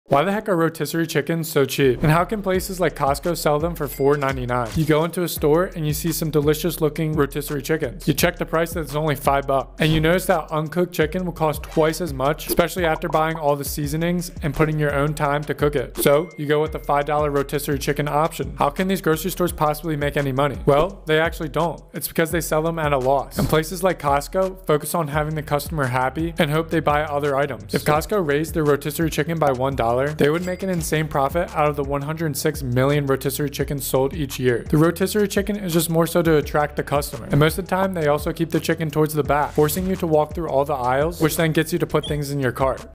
What the Cluck? Cheap rotisserie chicken
Entrepreneur looks at why Rotisserie chicken is sold so cheaply